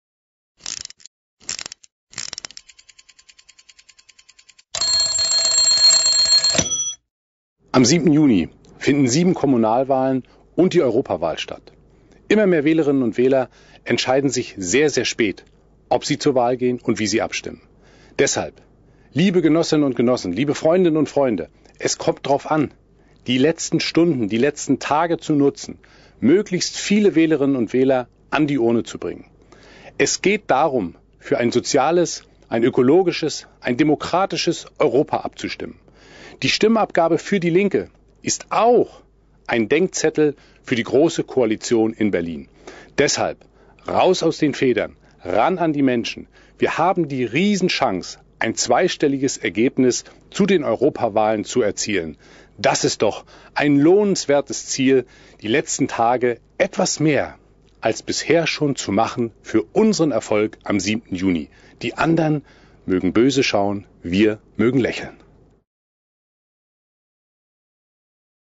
DIE LINKE, Dietmar Bartsch: Wählerinnen und Wähler an die Urne bringen!
Am 7. Juni finden 7 Kommunalwahlen und die Europawahl statt. Immer mehr Wählerinnen und Wähler entscheiden sich sehr spät, ob sie zur Wahl gehen und wie sie abstimmen. Es geht darum, für ein soziales, für ein ökologisches und für ein demokratisches Europa abzustimmen. Die Stimmabgabe für DIE LINKE ist auch ein Denkzettel für die Große Koalition in Berlin. Wir haben die riesige Chance, ein zweistelliges Ergebnis zu den Europawahlen zu erzielen. Nutzt die letzten Tage und Stunden, um möglichst viele Wählerinnen und Wähler an die Urne zu bringen!